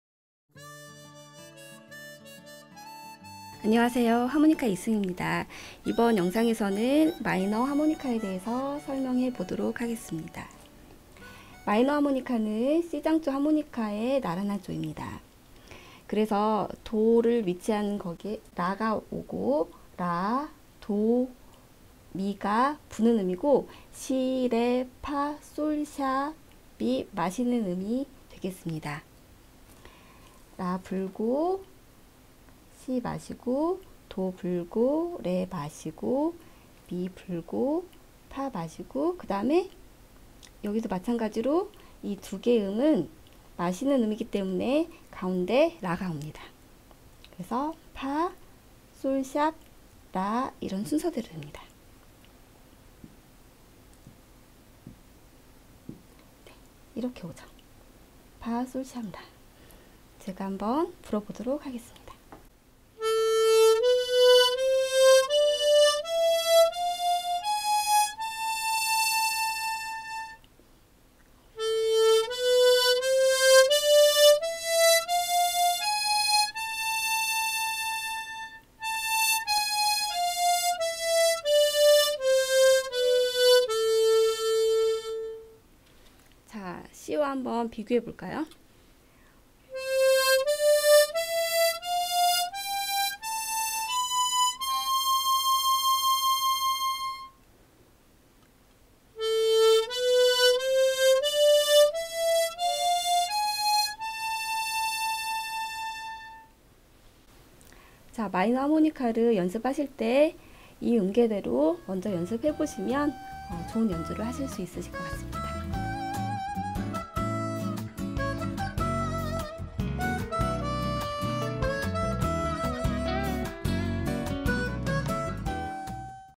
[자꾸만 하모니카] 12. 마이너 하모니카
현대음악출판사 "자꾸만 하모니카" 저자 이승희

트레몰로 하모니카는 메이저 하모니카와 마이너 하모니카로 나누어져 있습니다.

우리가 보통 연주하는 트레몰로 하모니카는 메이저 하모니카인데요,
이번 영상은 (마이너 하모니카)에 대한 설명입니다.